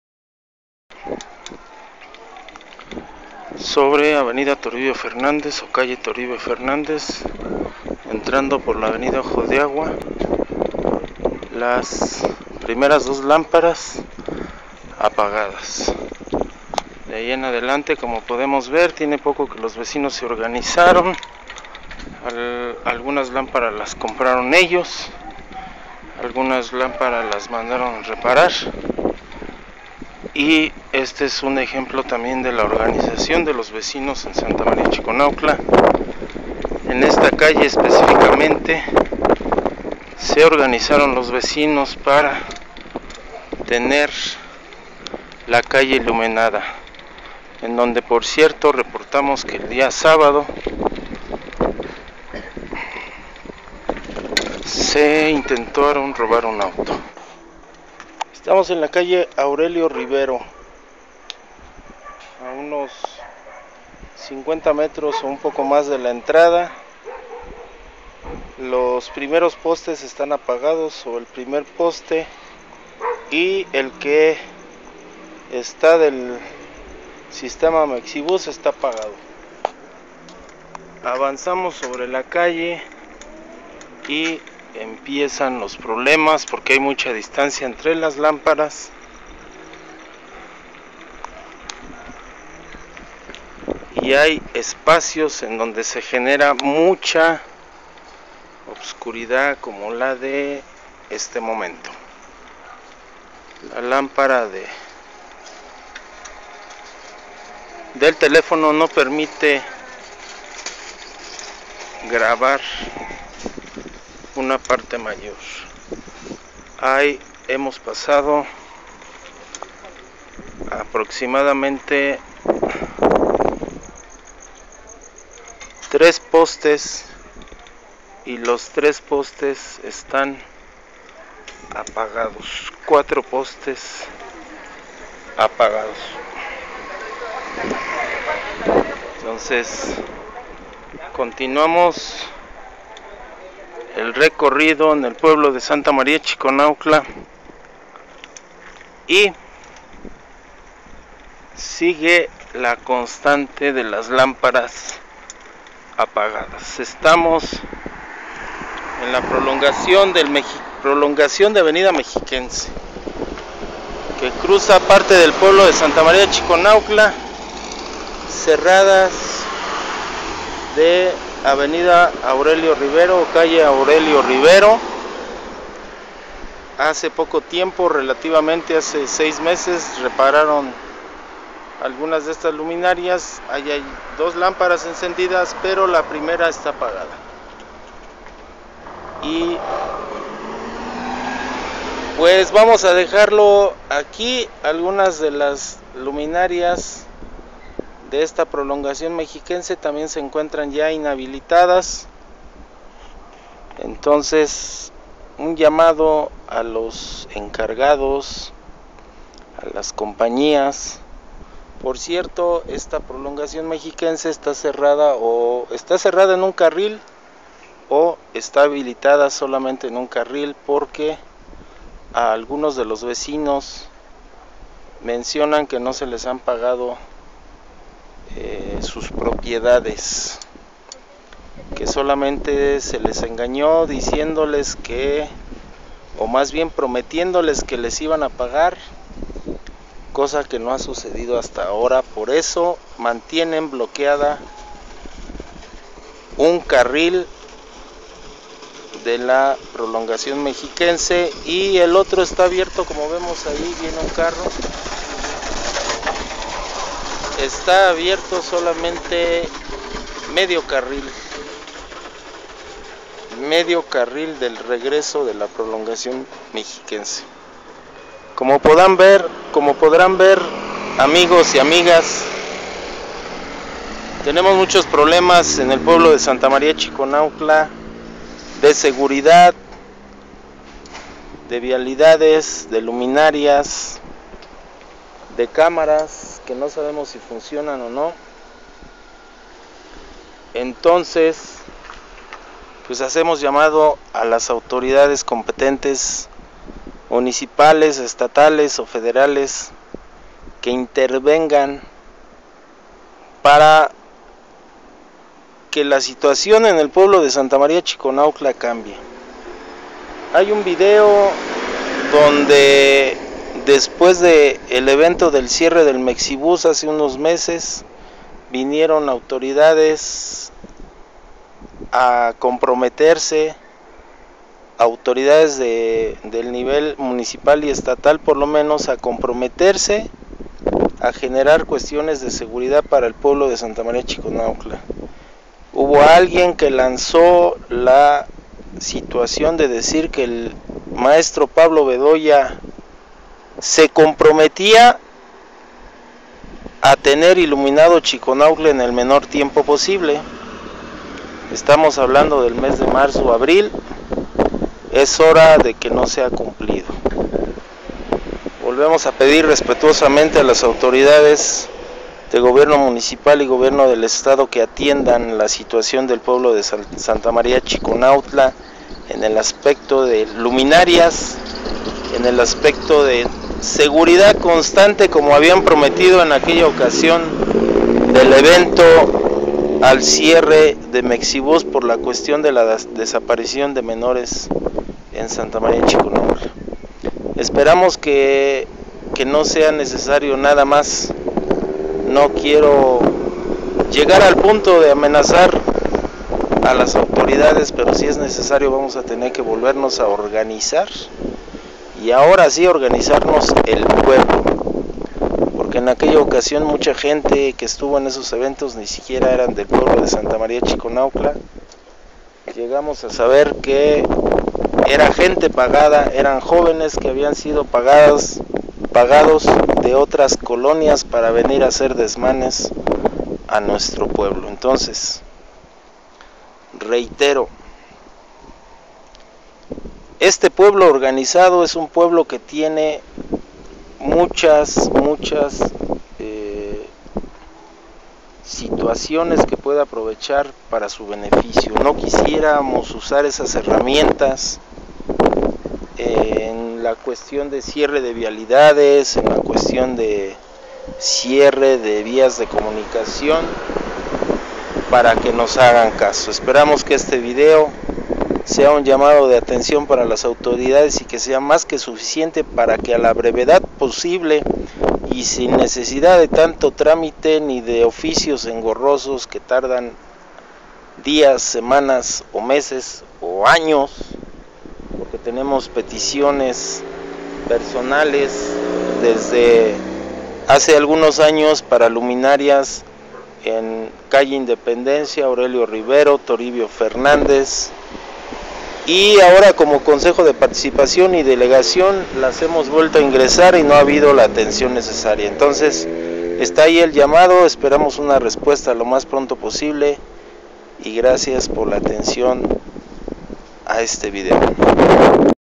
En total oscuridad vive el pueblo de Santa María Chiconautla en Ecatepec (3)
aquíEcatepec.- Santa María Chiconautla, uno de los nueve pueblos que integran el municipio de Ecatepec, por las noches es presa de la oscuridad por la ausencia de luminarias.

La falta de luz en casi toda la demarcación no es nada nuevo, y sí tolerada por muchas autoridades auxiliares durante los últimos 18 años, informa el primer Delegado de la comunidad, Roberto Sandoval Herrera.